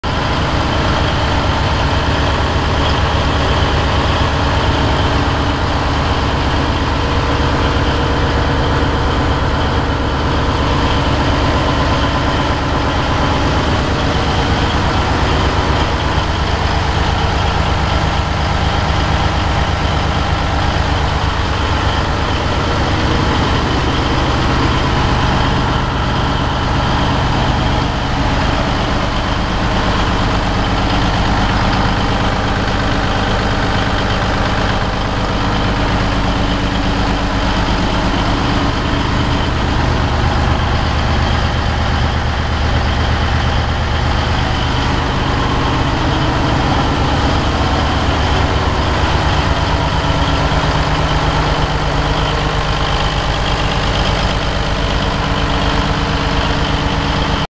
Caterpillar generator for sale at auction | bidding closes September 5, 2019
Caterpillar generator
Caterpillar 3406 six cylinder diesel engine
200 kW generator
It's located in Shell Knob, MO, and we're selling it to the highest bidder regardless of price.